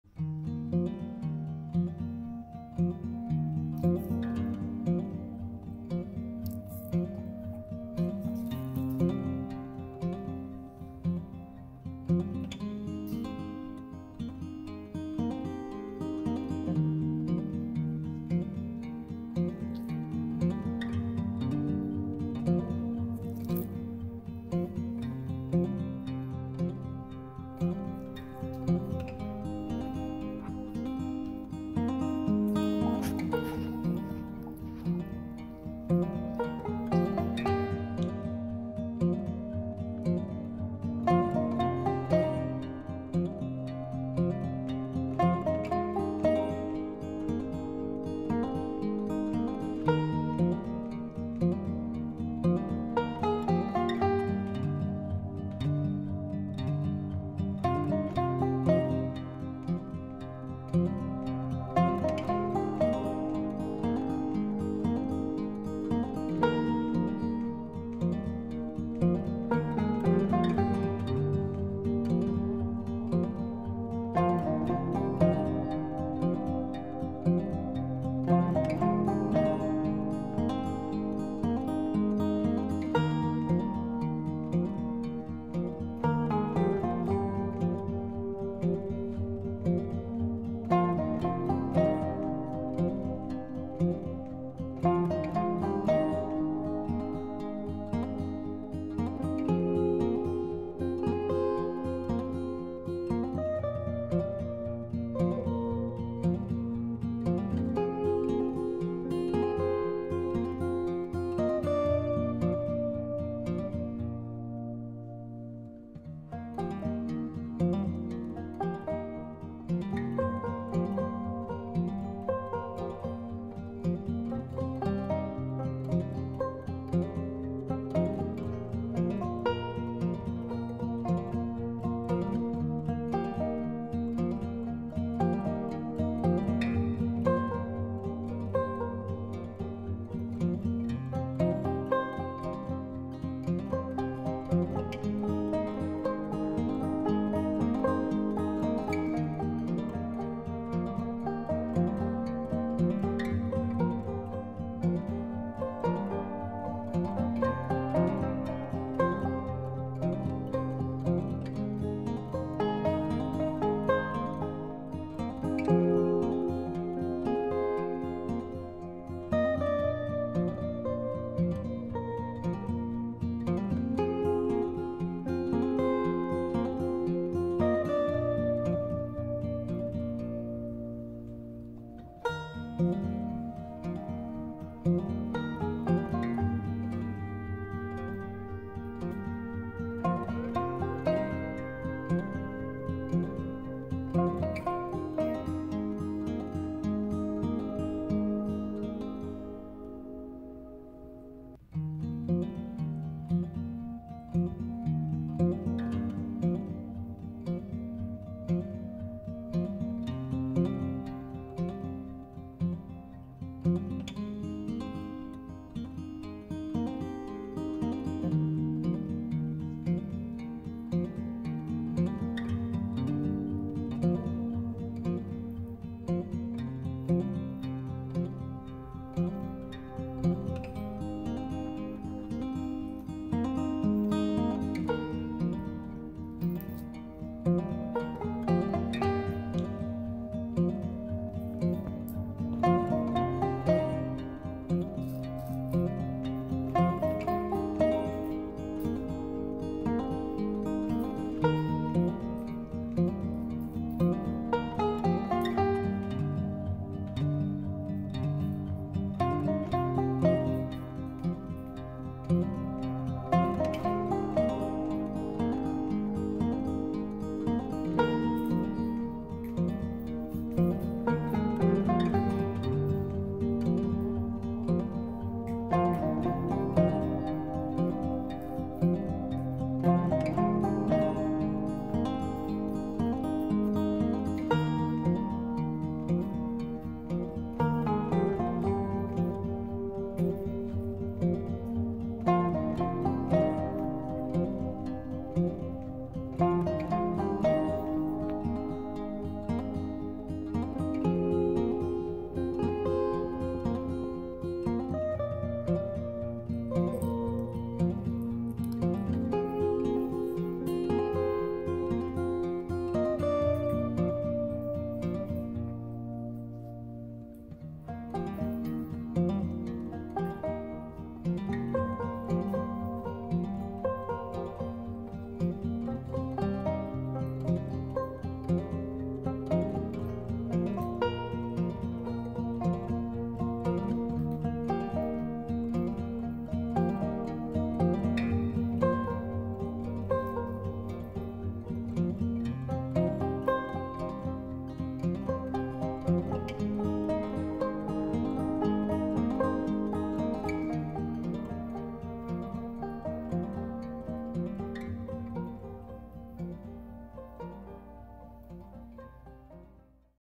Cats Trying Freeze-dried Raw Food From Nulo | Cats Review Nulo FreeStyle & MedalSeries
Cats Review Nulo FreeStyle & MedalSeries.
Featuring Kimi, Louise, Susie, Ginger Max, Fernando and Jack.